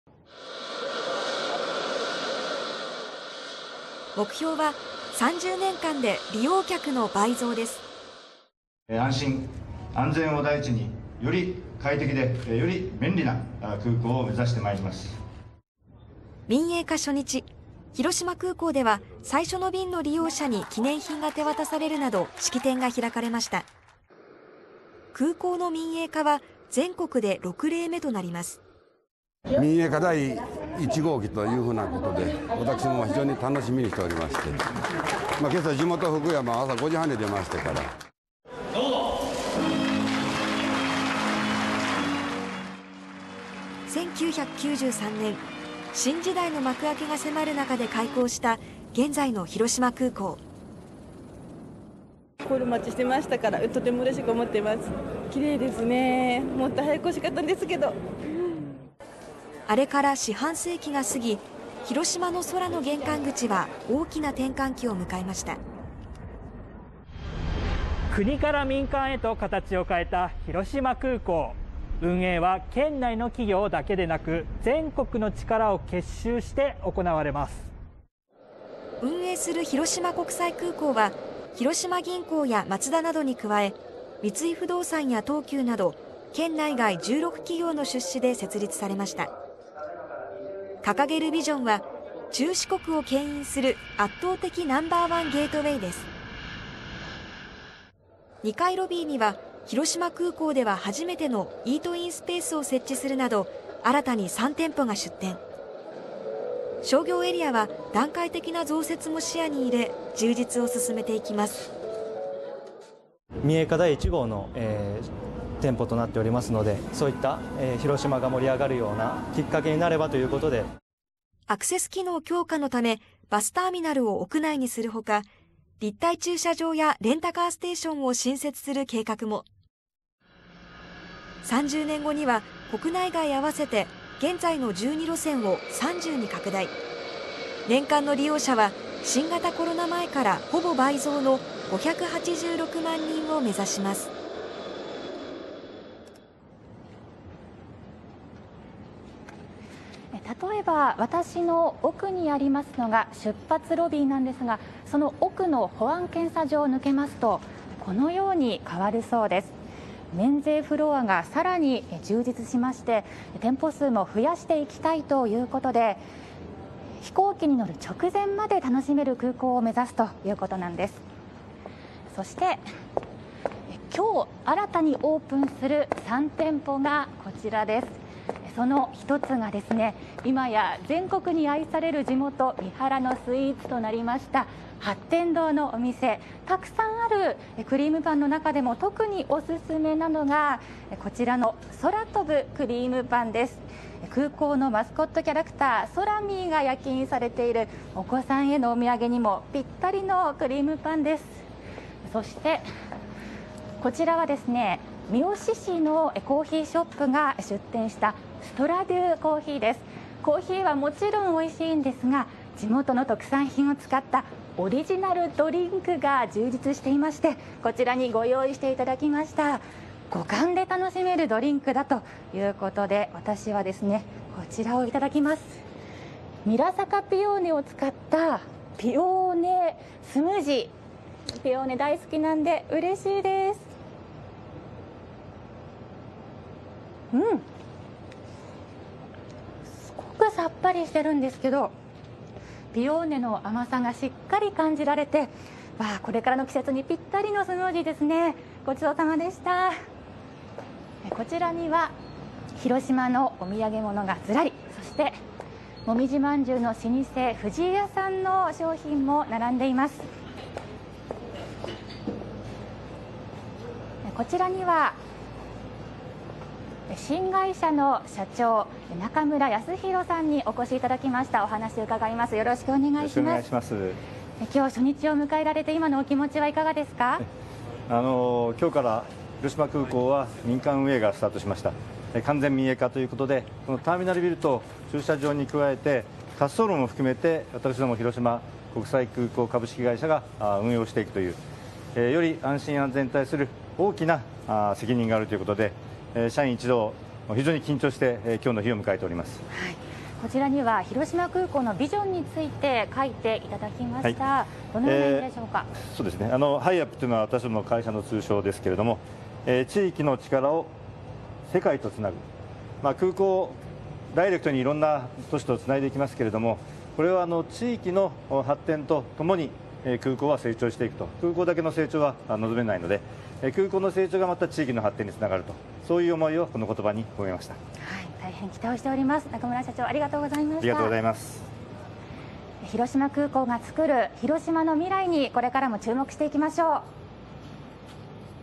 【広島空港が民営化】何が変わる？新店・グルメ紹介＆未来予想図【解説】
2021年7月1日より広島空港が民営化されました。
全国6例目の民営化で空の玄関口はどう変わるのか？
新店舗やグルメなど中継を交えて紹介します。

2021年7月1日放送